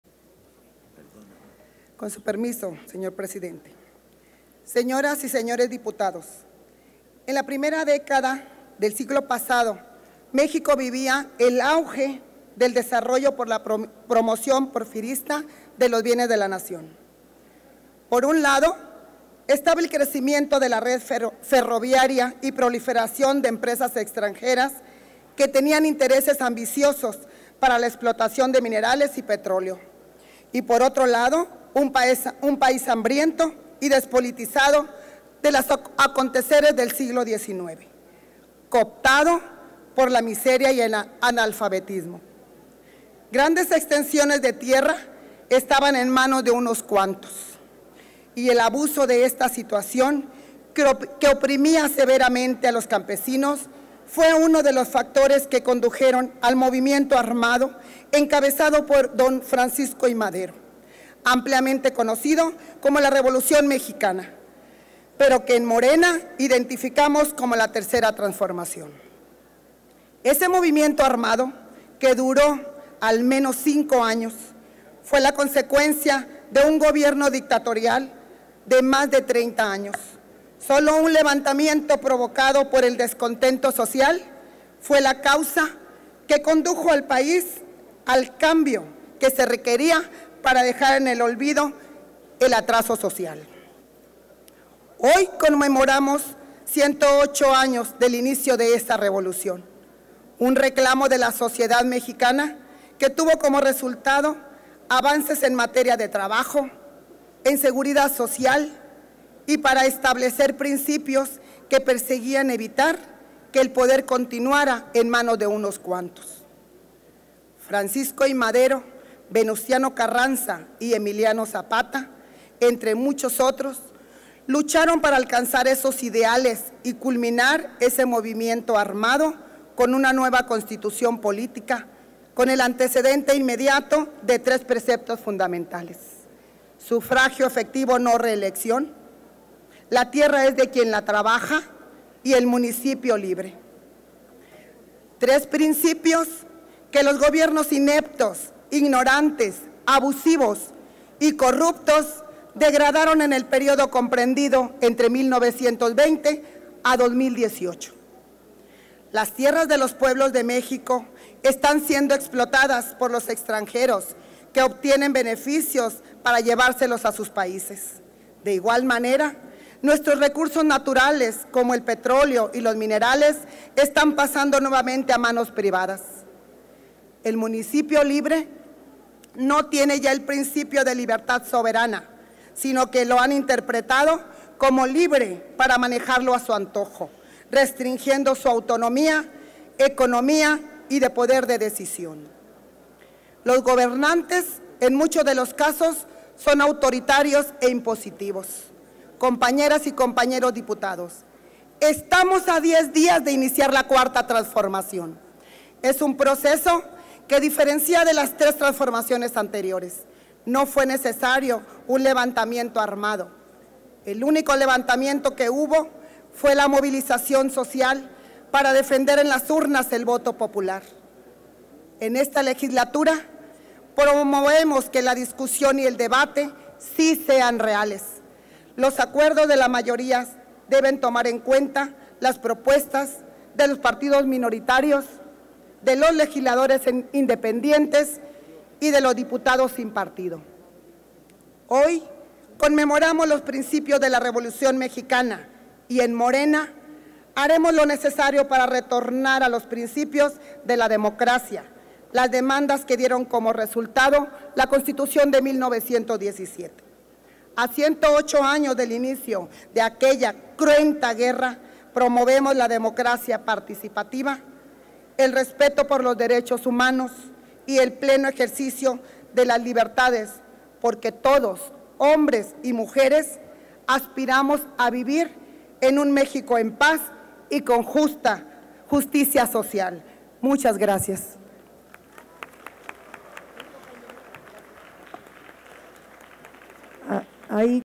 20/11/2018 Intervención en tribuna de la Dip. Olegaria Carrazco Macías
Intervención en tribuna de la Dip. Olegaria Carrazco Macías para hablar sobre la efeméride con motivo del -CVIII Aniversario de la Revolución Mexicana-.